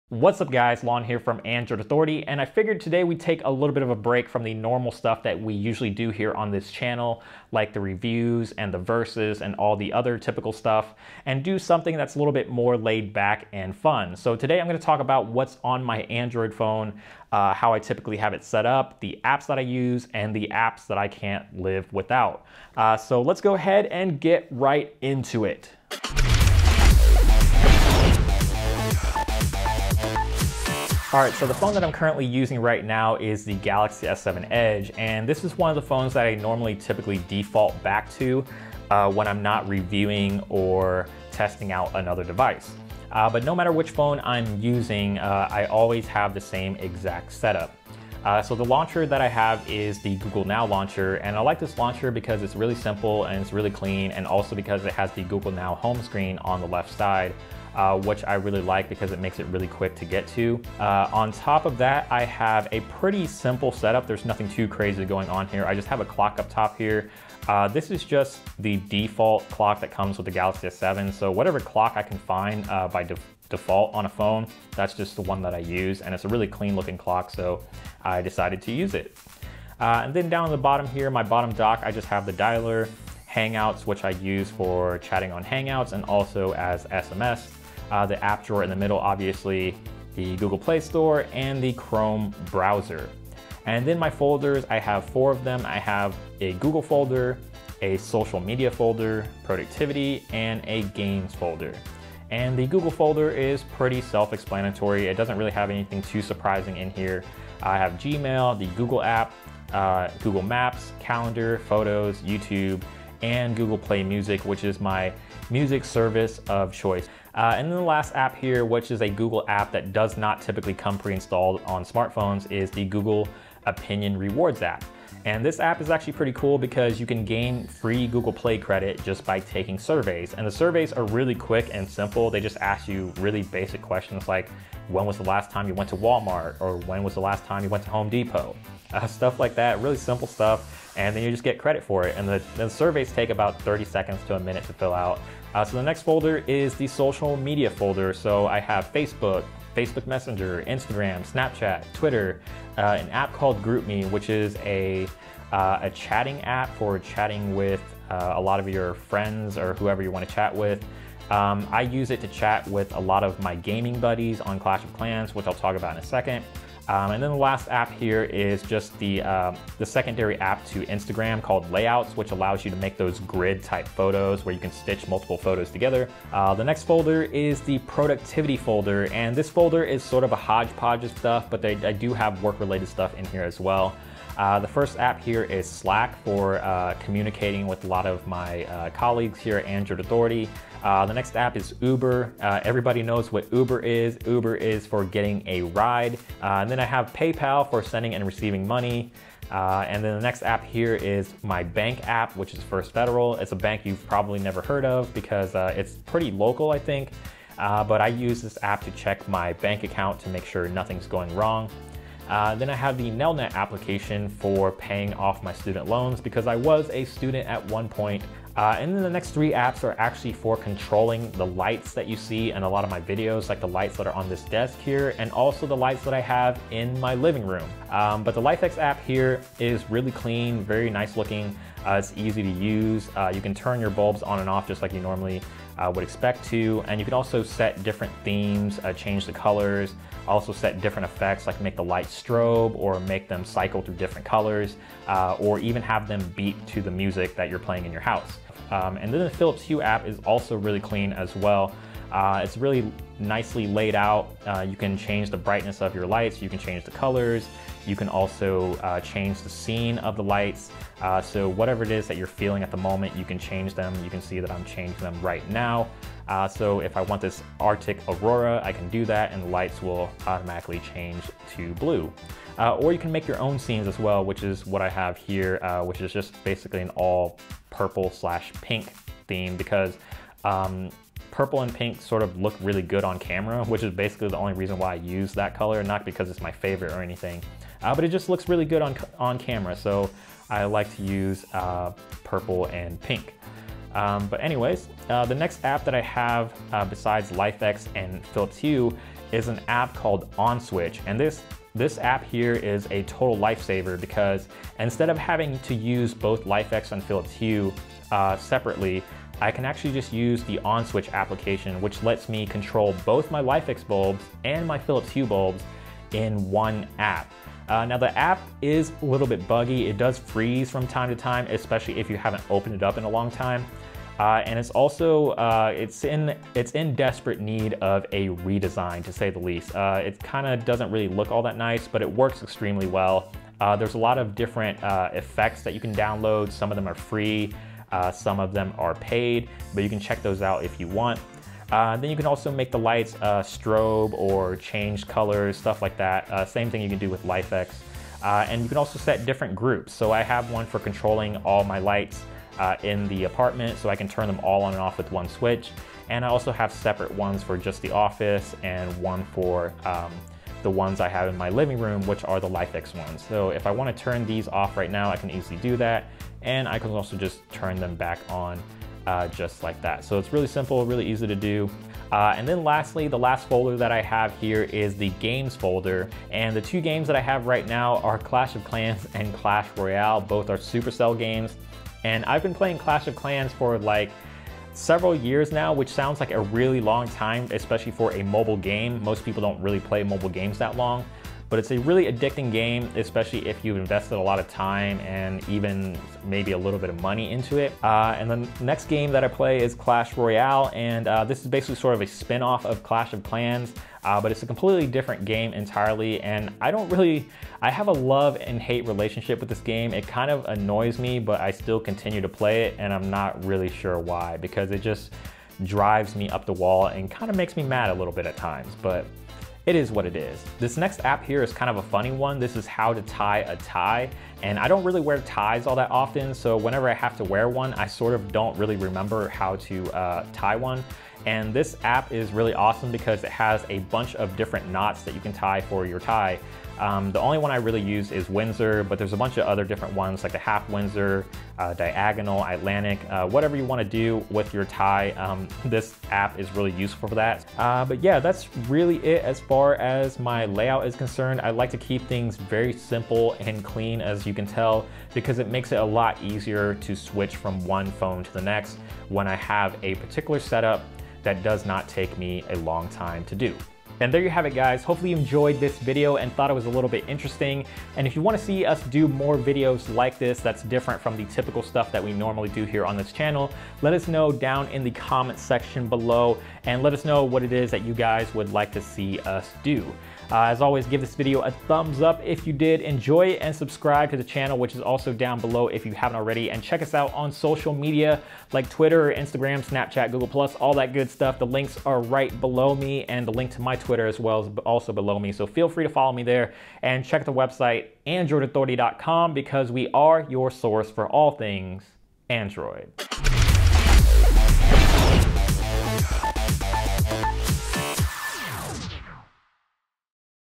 What's On My Android - My Setup and my must have apps
In the name of fun Lanh talks about how he usually sets up his Android device and the apps that he always has to have no matter what the device!